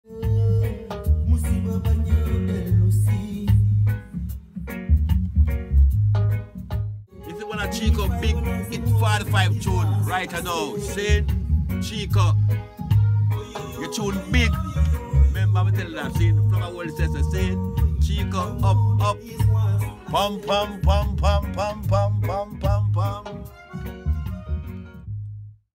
FLABBA HOLT ON MY UPCOMING SINGLE......STAY TUNED
Un grand merci à mon ami FLABBA HOLT Errol Carter, des ROOTS RADICS et ISRAEL VIBRATION, dont je suis un grand fan qui m'a fait l'honneur de poser sa basse sur mon prochain single. One love bredda.